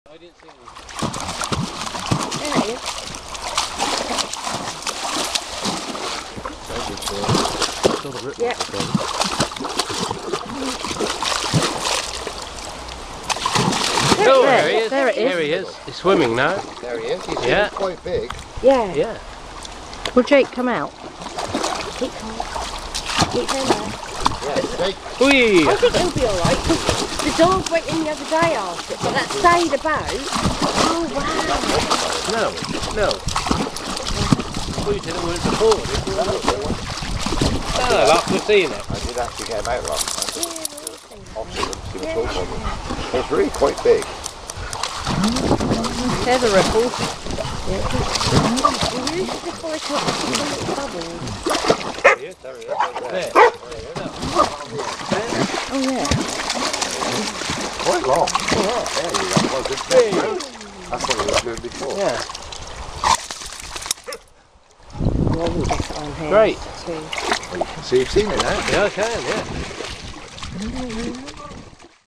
Very quick Otter sighting at Marston Marshes
A very quick sighting of the otter in the River Yare at Marston Marshes, Eaton, Norwich September 2012